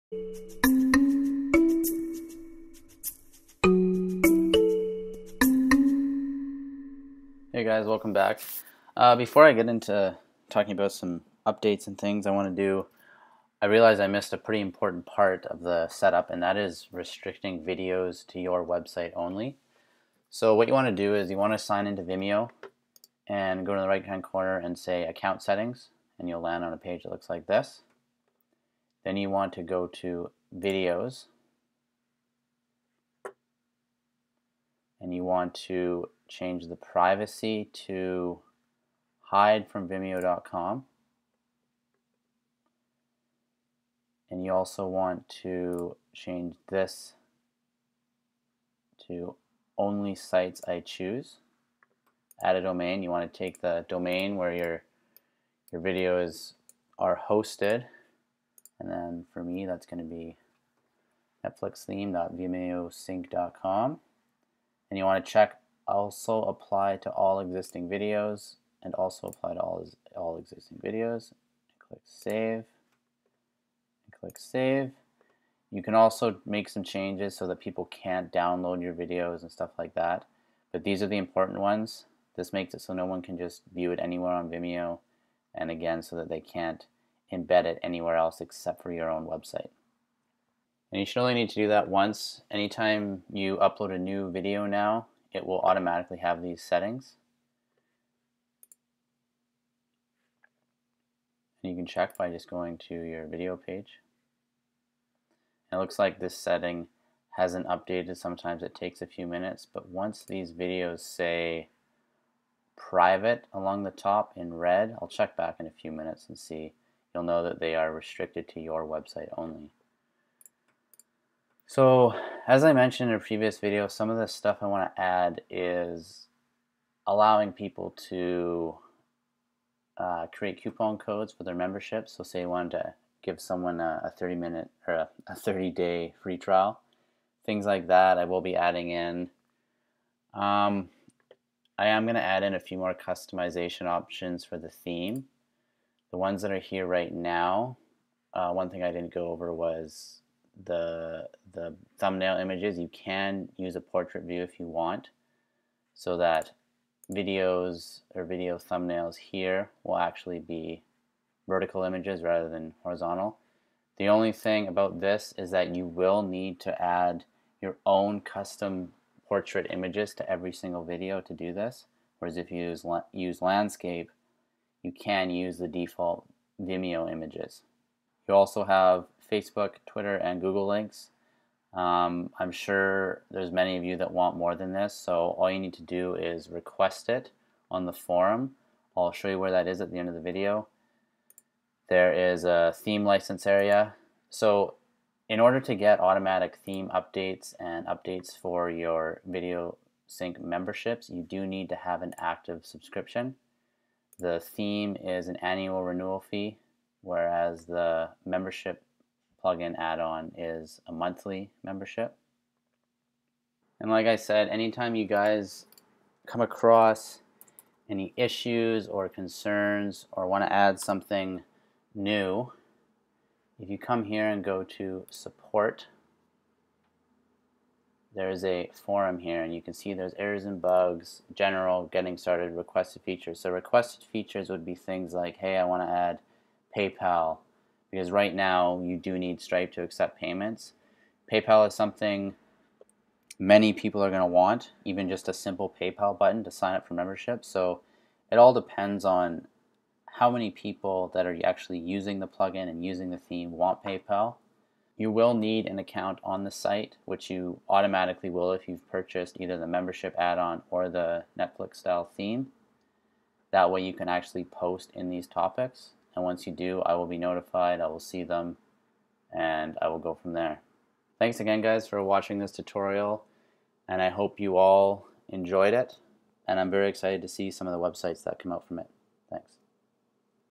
Creating Your Own Netflix - Restricting Videos & Updates
An important part missed was restricting your Vimeo videos to your WordPress website only. This video will cover that as well as mention some potential updates to come and how the forum works.